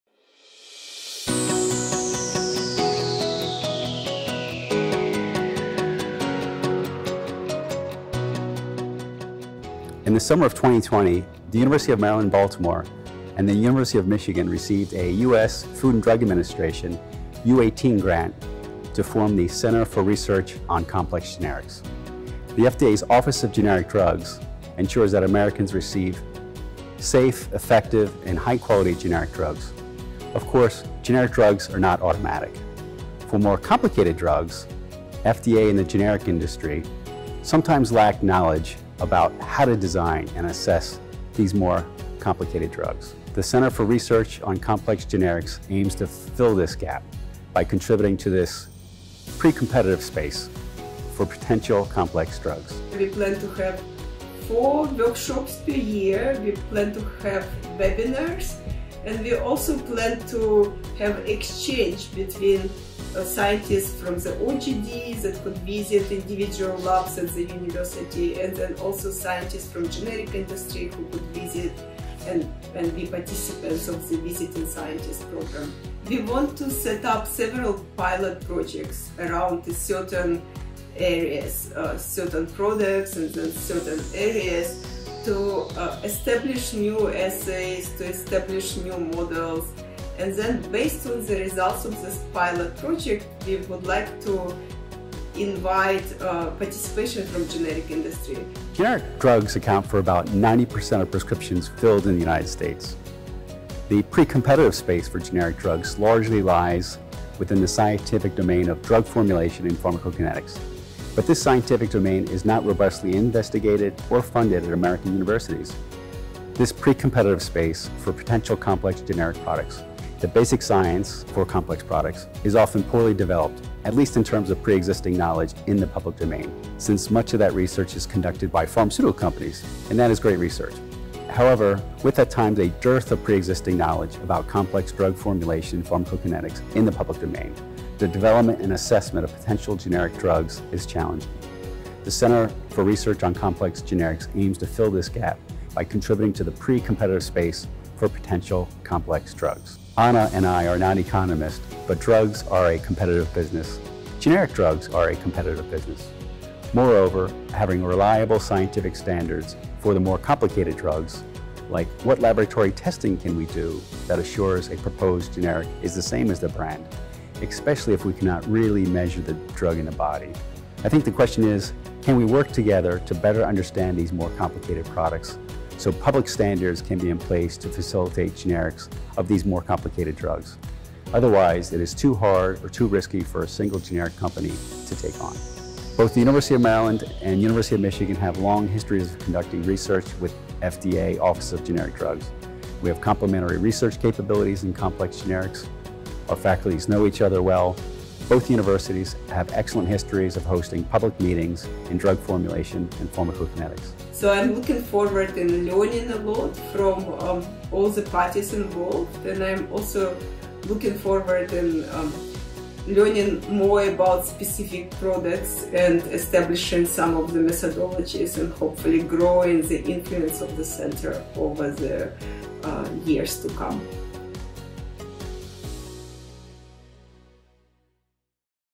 The Center for Research on Complex Generics
The Center for Research on Complex Generics aims to support the generic drug industry and other stakeholders by offering collaborative research and training through webinars, workshops, laboratory projects to meet development needs, and a Complex Generics Scholars program, among other initiatives. This first of its kind, cutting-edge center will stimulate innovative dialogue, disseminate current understanding of complex products and practices, and generate new knowledge in support of FDA’s mission to promote and protect the public health by increasing access to safe and effective generic medicines.